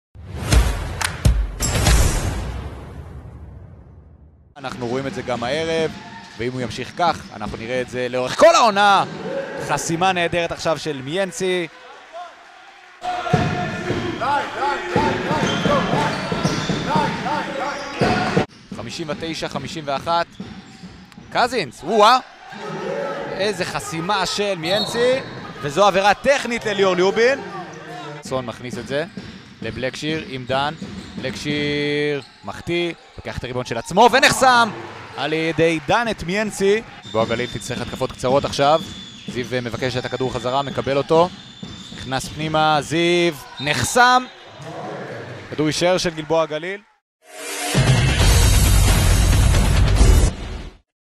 Jerome Meyinsse Blocks in Irony Ness-Ziona vs. Hapoel Galil-Gilboa
Jerome Meyinsse Blocks in Irony Ness-Ziona vs. Hapoel Galil-Gilboa, 12/24/2020